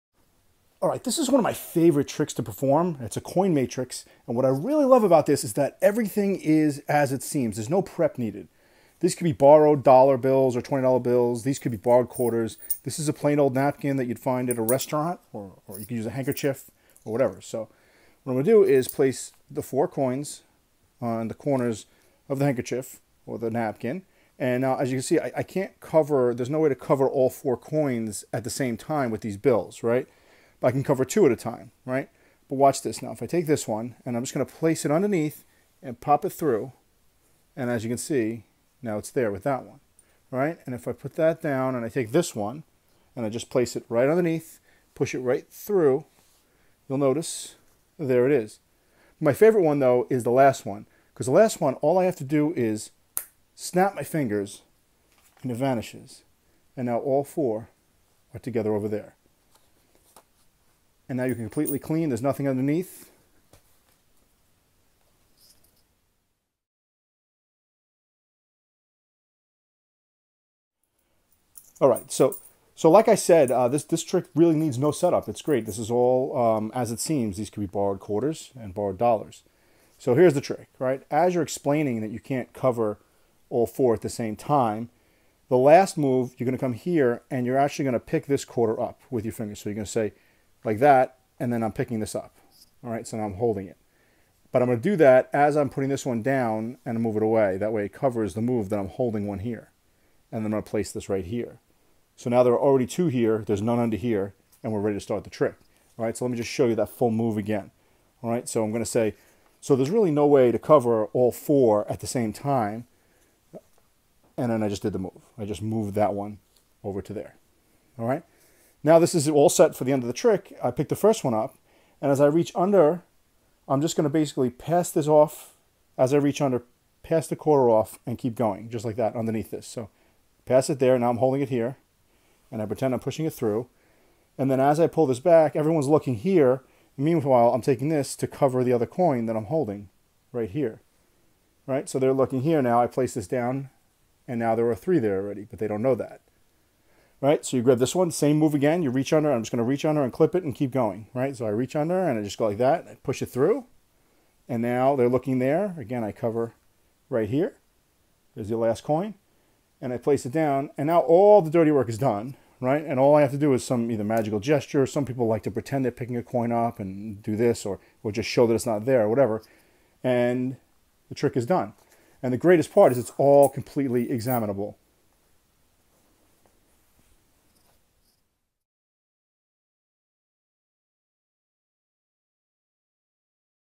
Awesome Coin Matrix Trick that is super easy!
Coin Matrix - Secret Revealed - This video teaches a simple method to perform this awesome magic trick that looks like real magic!